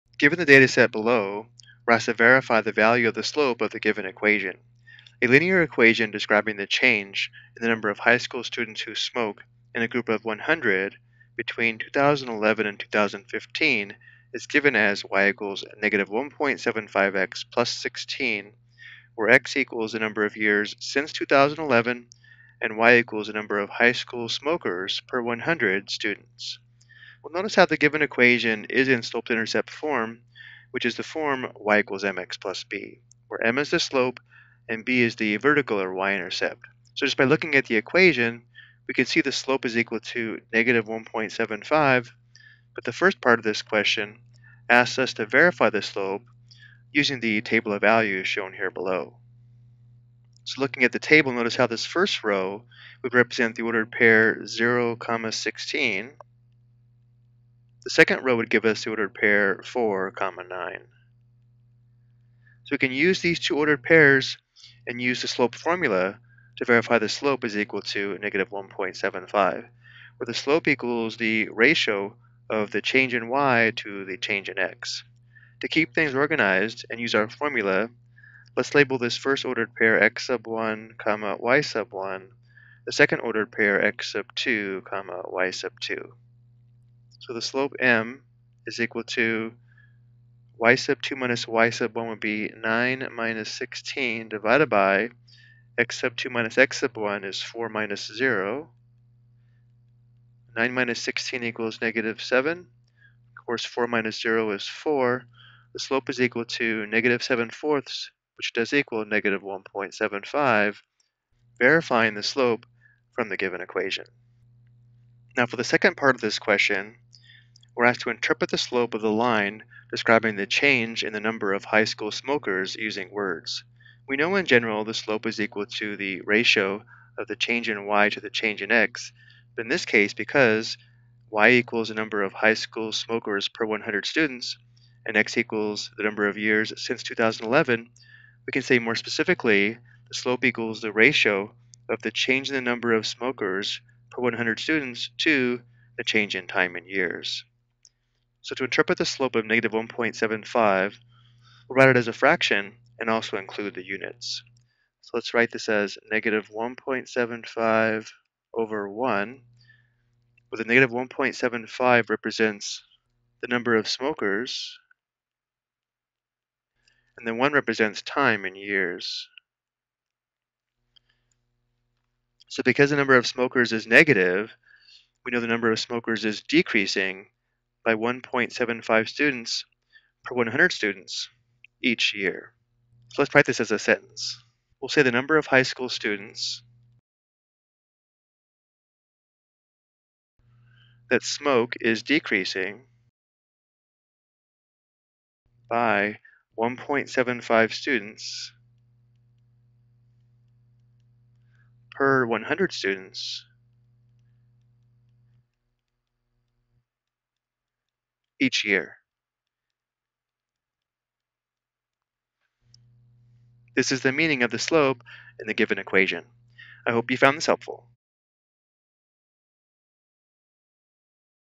Interpret the Meaning of the Slope of a Linear Equation - Smokers
This video explains how to interpret the meaning of slope given a  linear equation.